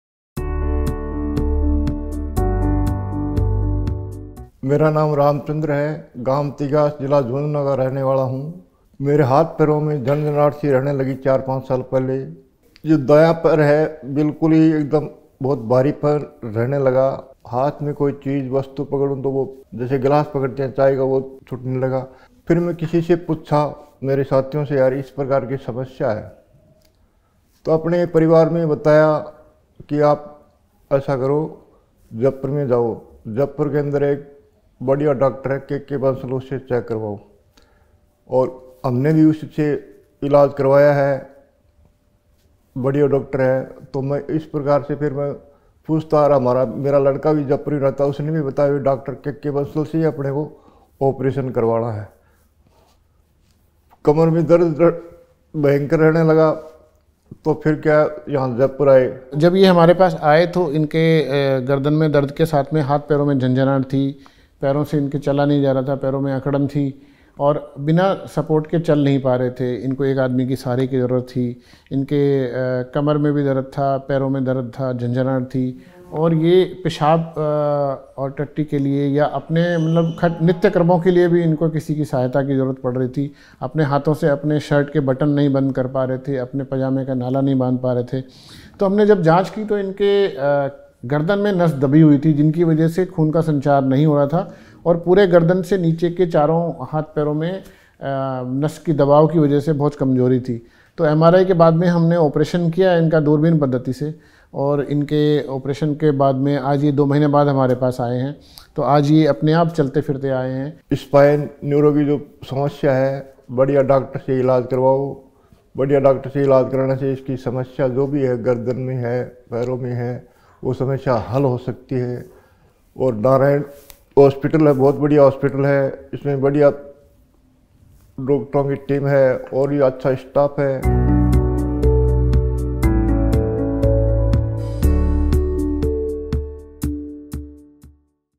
Patient Success Story of RamChandra Dr KK Bansal
Mr. Ram Chandra was experiencing pain and extreme tingling sensation in his hands and legs. He shared this issue with his friends and family. He was recommended to Dr KK Bansal, Senior Consultant-Neurosurgery, Narayana Multispeciality Hospital, Jaipur.

The doctor discovered:
Stiffness in his legs
Pain in the lower back
Difficulty in Movement
Neck pain
Difficulty moving without support.
Needed help with all the movements and activities.

The main cause was a pinched nerve in his neck which caused all of this. It led to a restricted flow of blood that affected the movement of his legs and hands. After the MRI of the patient, spine surgery was performed, which yielded satisfactory results.